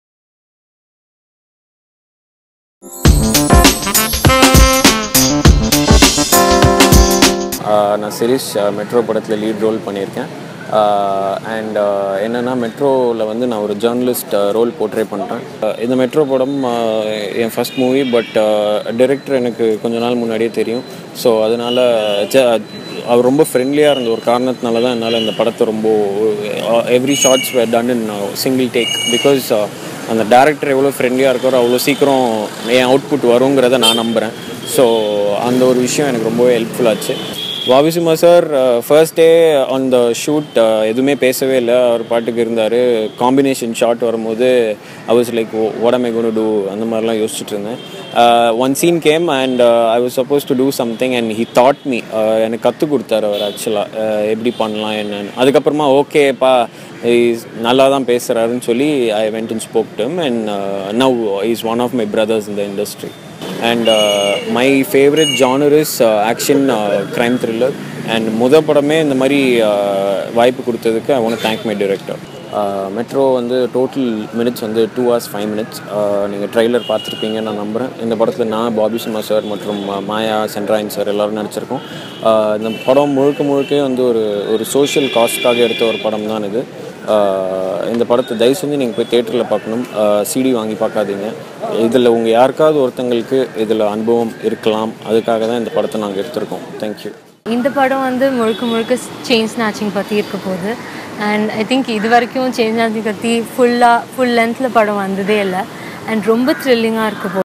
Shirish about Bobby Simha & Metro Tamil Movie | Exclusive Interview | Maaya
Actor Shirish about Bobby Simha & Metro Tamil Movie. Shirish Saravanan Exclusive interview on Thamizh Padam about Metro Movie ft. Bobby Simha, Shirish, Sendrayan, Sathya, Nishanth, Rajkumar and Yogi Babu. Johan composed Music.

Metro Movie is Directed by Anandakrishnan. Produced by Jaya Krishnan and Anandakrishnan under E5 Entertainments and Metro Productions.

Metro Tamil Movie Cast & Crew :

Cast : Shirish, Bobby Simha, Sendrayan, Maaya, Sathya among others.
Director : Ananda krishnan
Producer : Jaya Krishnan, Anandakrishnan
Written by Anandakrishnan
Music Director : Johan
Cinematography : N. S. Uthaya Kumar
Edited : M. Ramesh Baarathi